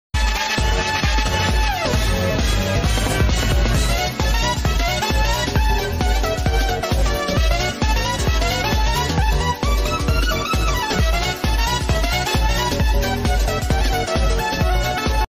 You're Hired (stock photo meme)
when you get hired and you become a 40 variants of stock photos and dance